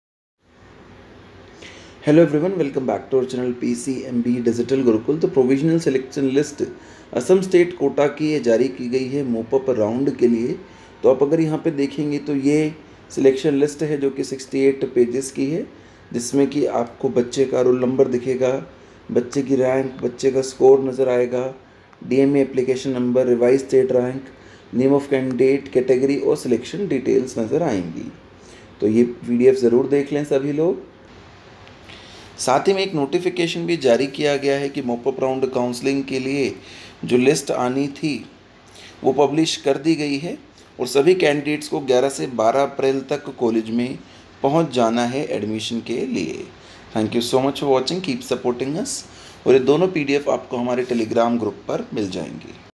Assam mop up round MBBS BDS allotment list category wise NEET UG 2021 | PCMB DIGITAL GURUKUL
Hello everyone, thanks for watching our video in this video, I've explained in detail about Assam mop up round MBBS BDS allotment list category wise NEET UG 2021 | PCMB DIGITAL GURUKUL